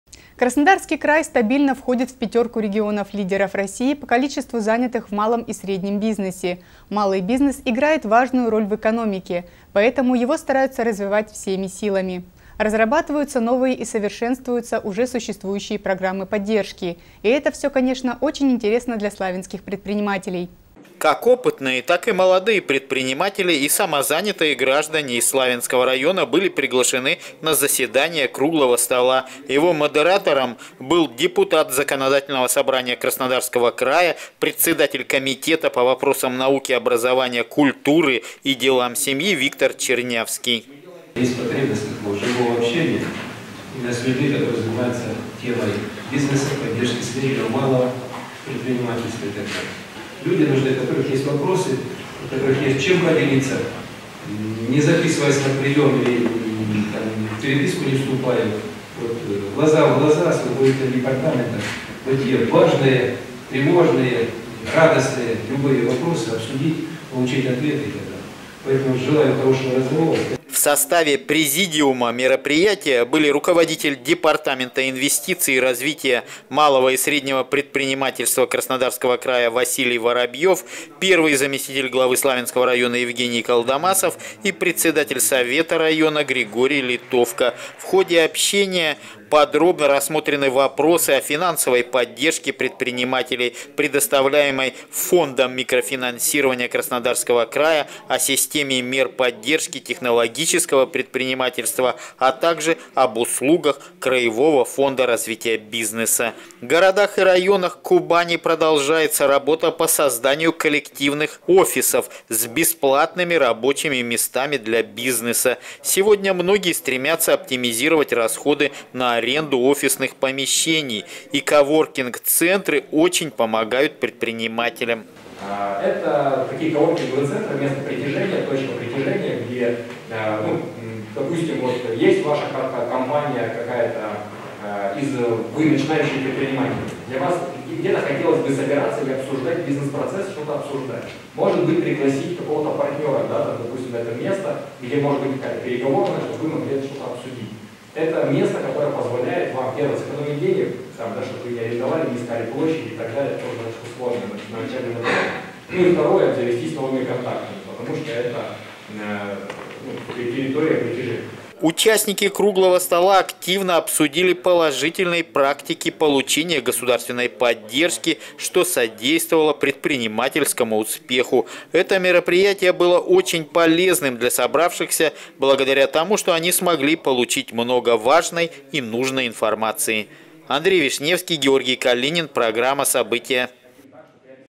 Поддержка для предпринимателей
Краснодарский край стабильно входит в пятерку регионов-лидеров России по количеству занятых в  малом и среднем бизнесе. Малый бизнес играет важную  роль в экономике, поэтому его стараются развивать всеми силами. Разрабатываются новые и совершенствуются уже существующие программы поддержки. И это все, конечно, очень интересно для  славянских предпринимателей